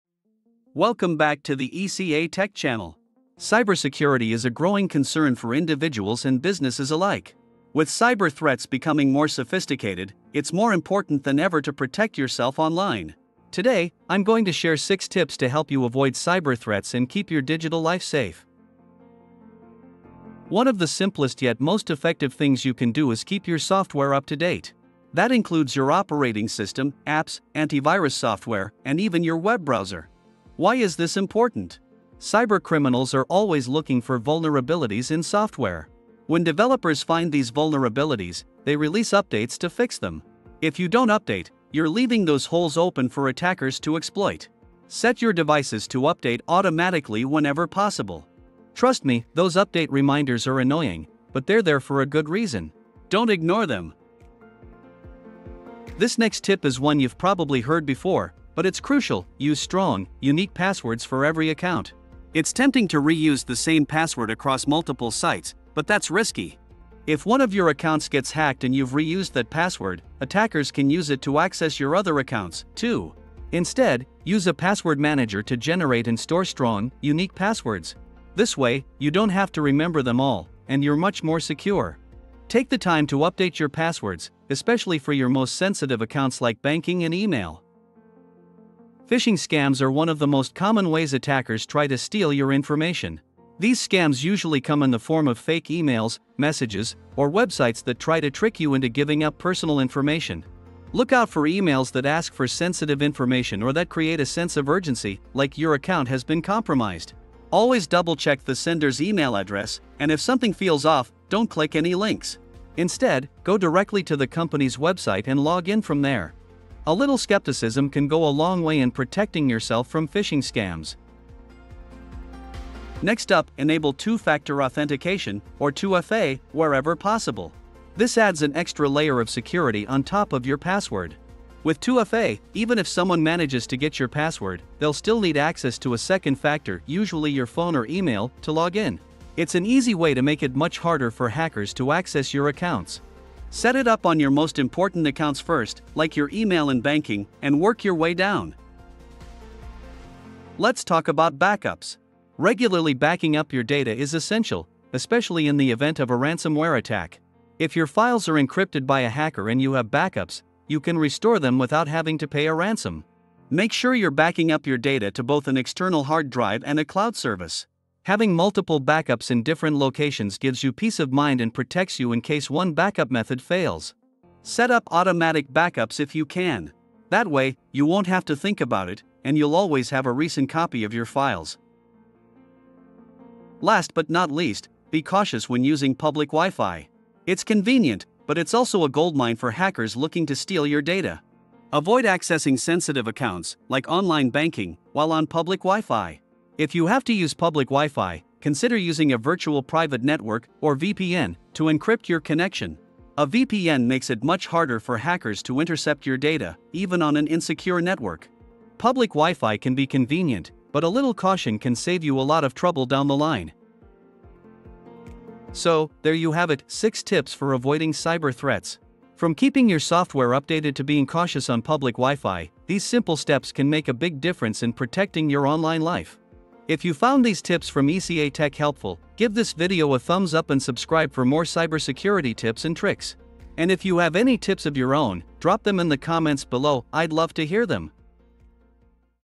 6 Tips for Avoiding Cyber Threats
In today's digital age, protecting yourself from cyberattacks is more important than ever. This video will provide you with practical advice to safeguard your personal and professional information.

Key topics covered:

Strong passwords: Learn how to create strong, unique passwords that are difficult to hack.
Phishing scams: Understand the common tactics used by phishers and how to spot and avoid them.
Public Wi-Fi: Learn the risks of using public Wi-Fi and how to protect your data when connected to unsecured networks.
Regular updates: Keep your software and operating system up-to-date to patch security vulnerabilities.
Two-factor authentication: Discover how to add an extra layer of security to your online accounts using two-factor authentication.
Backups: Learn the importance of regularly backing up your data to protect against data loss in case of a cyberattack.
Follow these simple steps to stay safe online and protect your valuable information.